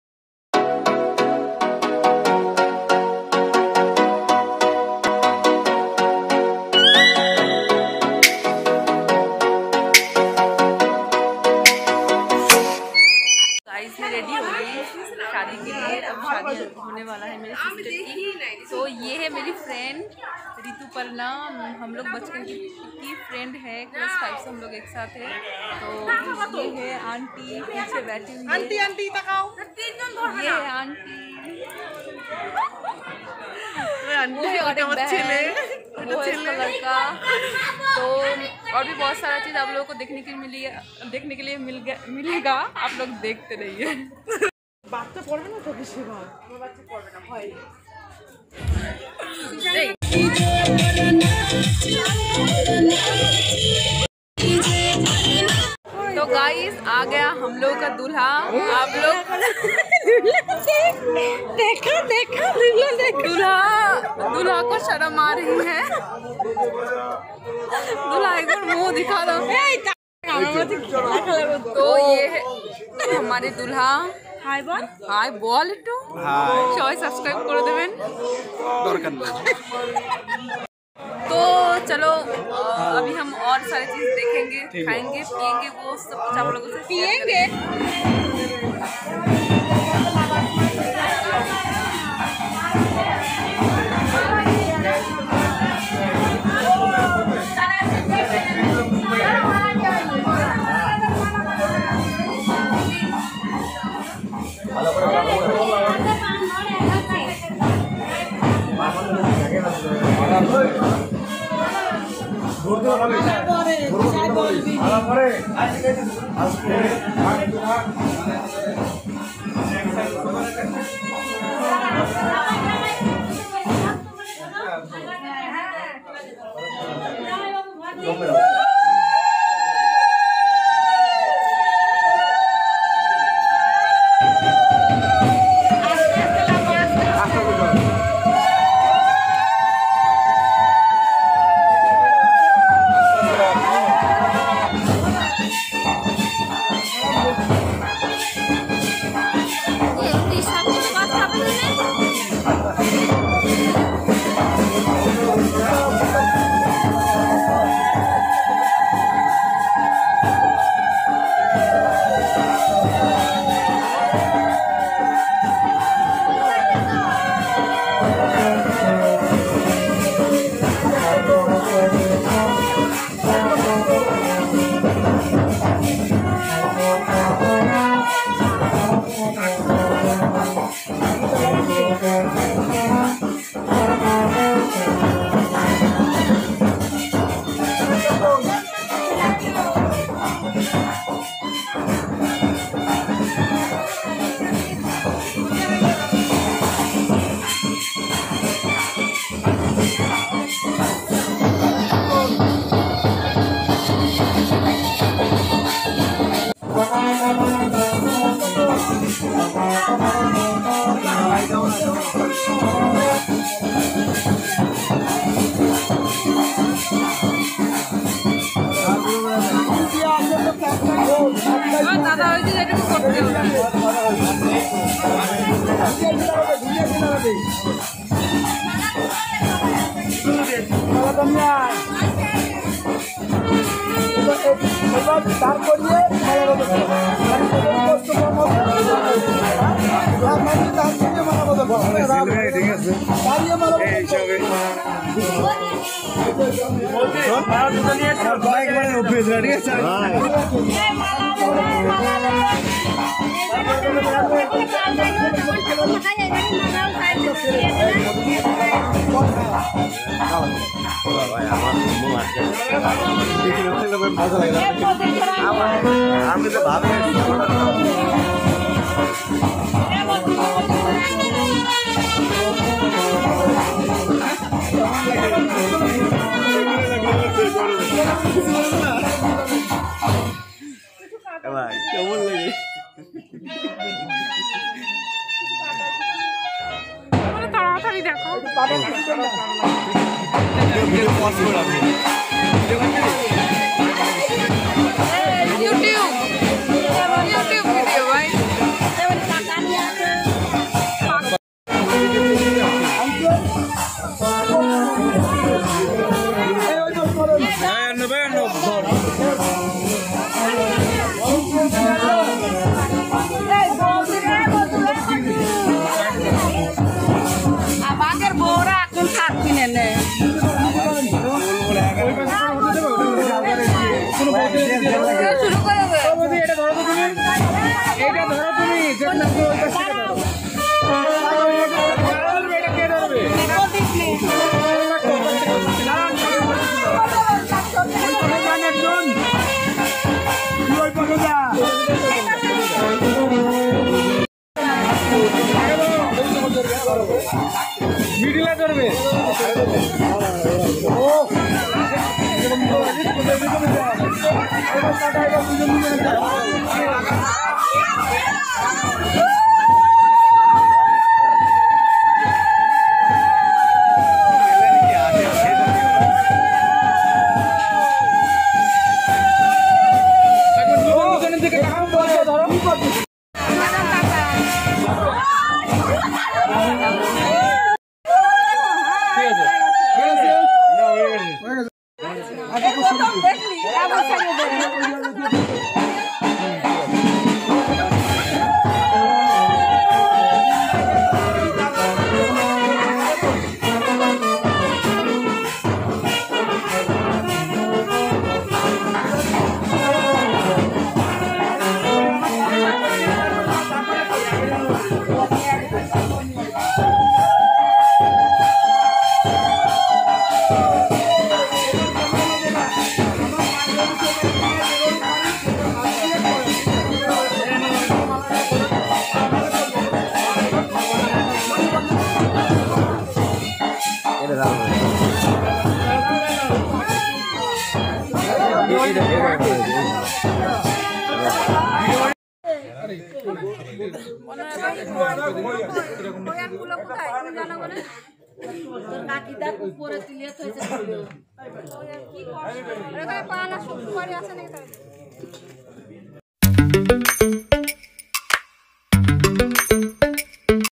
Bengali traditional marriage part-1💏||Mala badal😍|suvo dristi😘supriya lifestyle 🥰❤️❤️
Traditional Bengali  marriage part-1

my sister marriage

suvo dristi

mala balad

thx u so much🥰